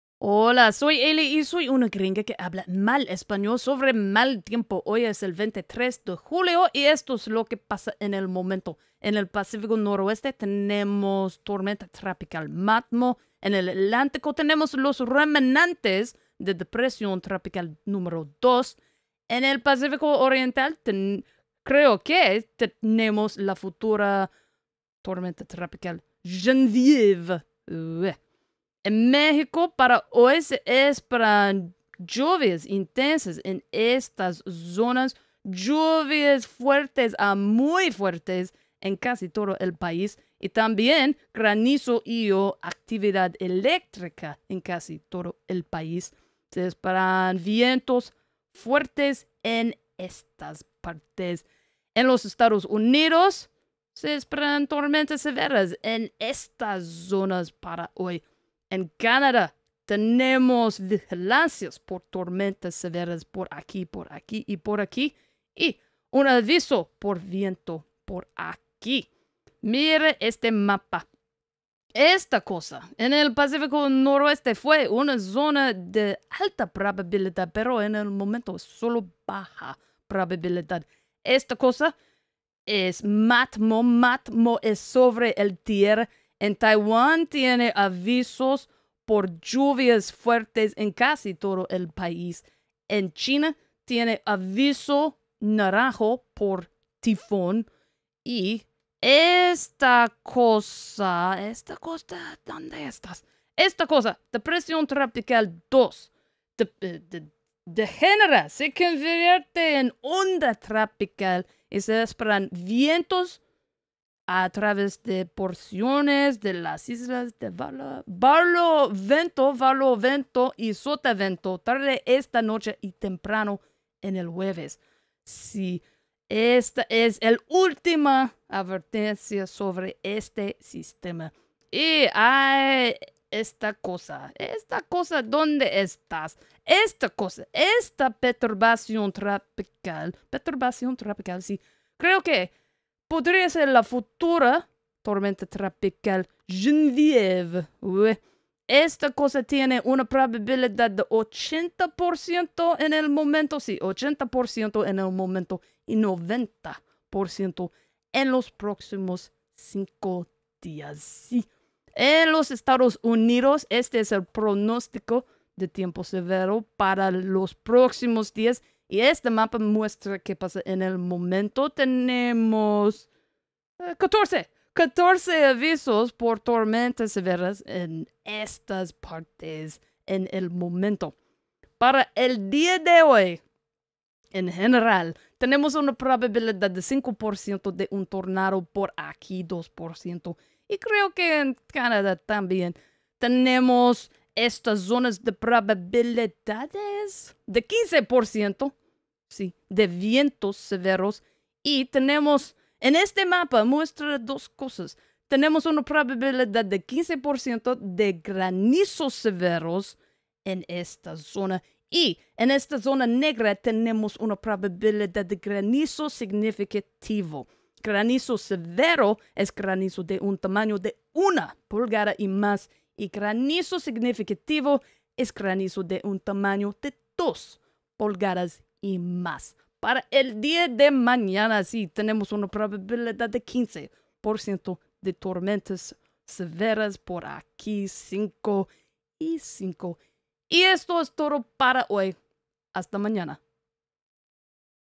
23 de julio - Tormenta Tropical Matmo, DT #2 se convierte en una onda tropical
# I am hayley
# soy "jeli"

# severe weather outlook
## 23 de julio / July 23

# What's up? / ¿Qué pasa?

## cyclones / ciclones

* Northwest Pacific / Pacífico Noroeste: *Matmo*
* Atlantic / Atlántico: *TD #2*
* Eastern Pacific / Pacífico Oriental: *Genevieve*

Next storm names: Eastern Pacific: *Genevieve*, Atlantic: *Bertha*

## México

## US / EE.UU.

## Canada / Canadá

# cyclocane
.com

# severe weather outlook
.com

# today/hoy

# tornado

# wind/viento

# hail/granizo

# tomorrow/mañana